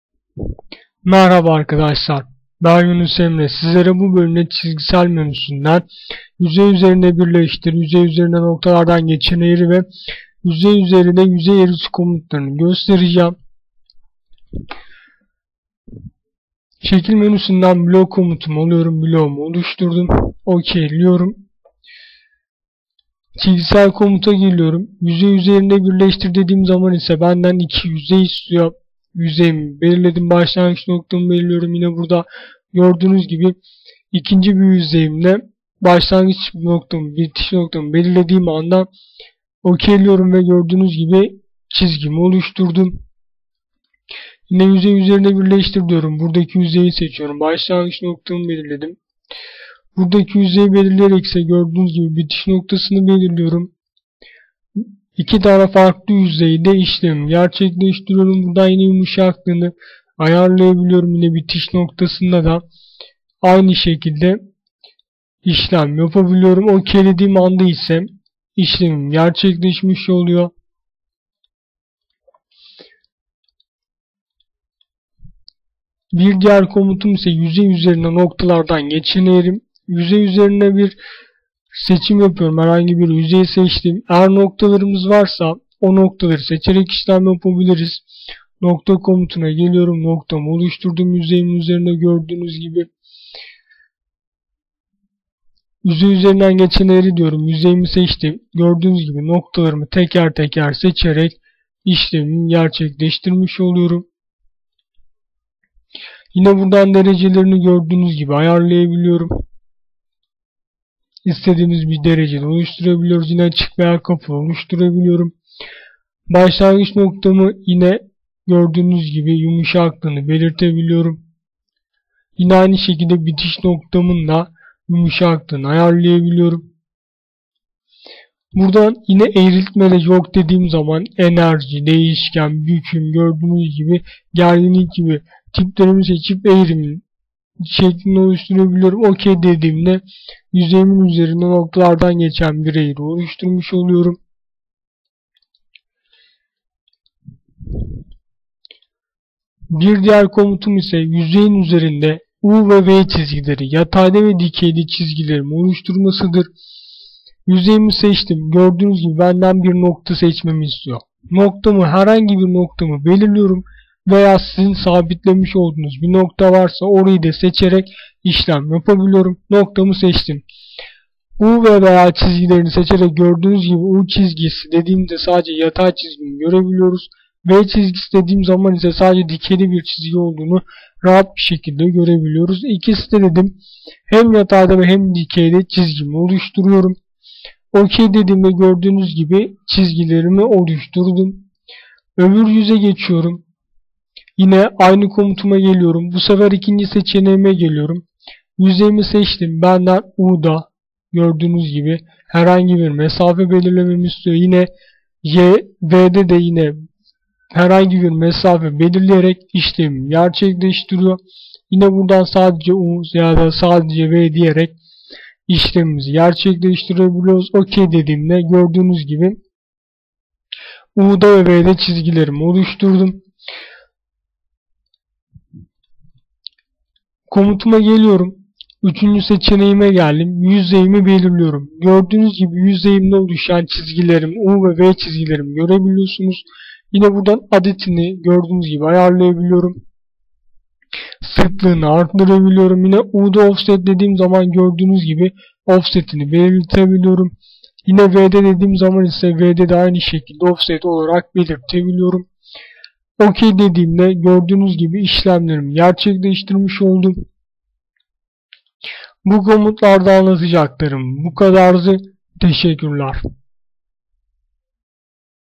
009 YÜZEY ÜZERİNDE BİRLEŞTİR,YÜZEY ÜZERİNE NOKTALARDAN GEÇEN EĞRİ,YÜZEY ÜZERİNDE YÜZEY EĞRİSİ
ZW3D Türkçe CAD/CAM Yazılımıdır.

Ücretsiz olarak 30 gün full versiyon olarak deneyebilirsiniz.

Demo talebi için ziyaret edebilirsiniz. yada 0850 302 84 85 arayıp demo talebinde bulunabilirsiniz.